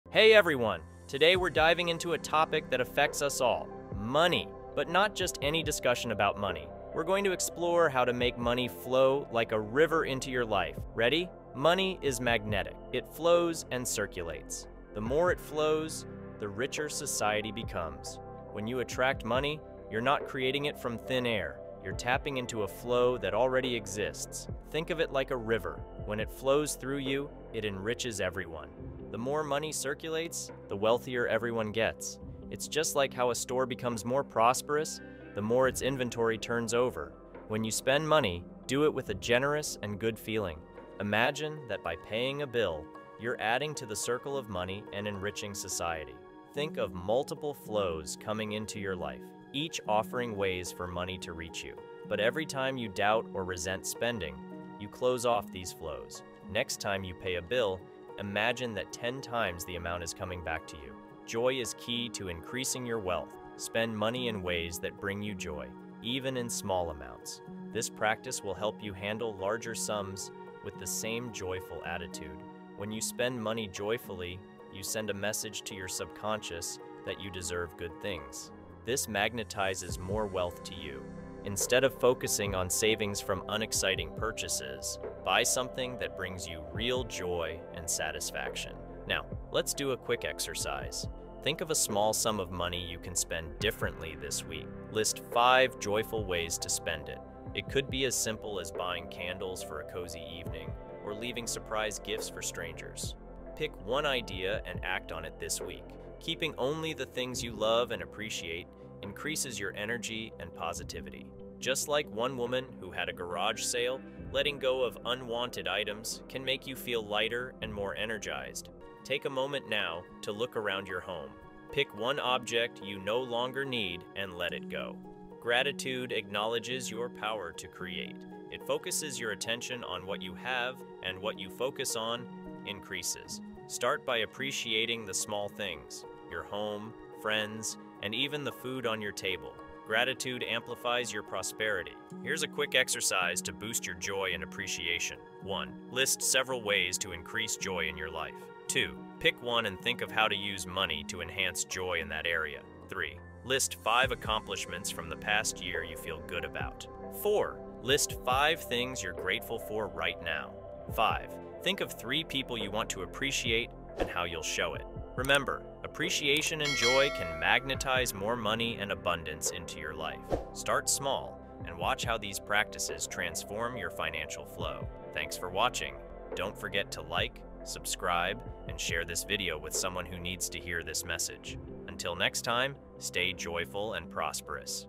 How to Create Money by Cultivating Joy and Aliveness
Joy and aliveness are powerful magnets for financial abundance. In this video, we explore how cultivating these positive emotions can enhance your ability to attract wealth. Learn practical strategies for bringing more joy into your life, from daily practices to mindset shifts, and discover how this energy can open the flow of prosperity. By living joyfully, you can create money more effortlessly and enjoy the process.

Link for a full book